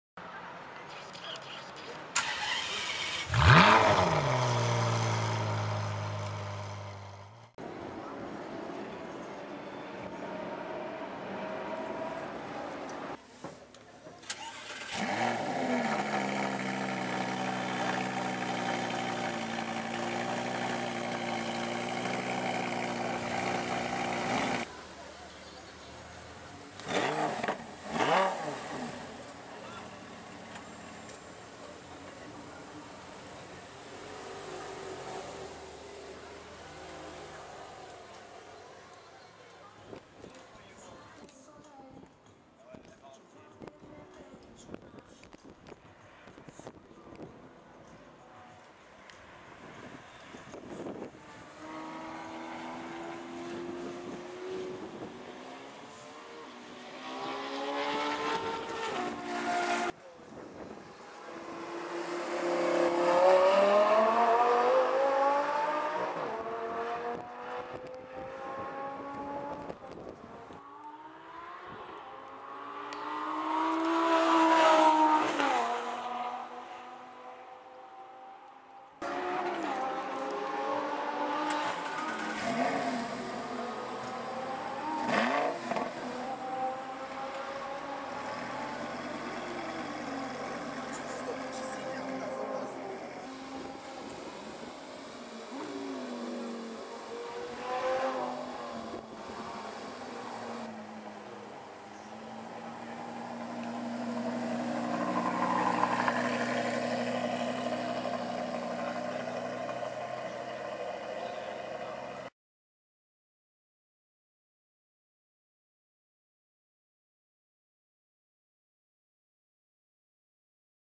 Blue Lamborghini Gallardo LP570 Superleggera Start, Revs & HARD Acceleration! (HD)
Lamborghini Gallardo LP570 Superleggera at the Circuit Paul Ricard HTTT (Le Castellet)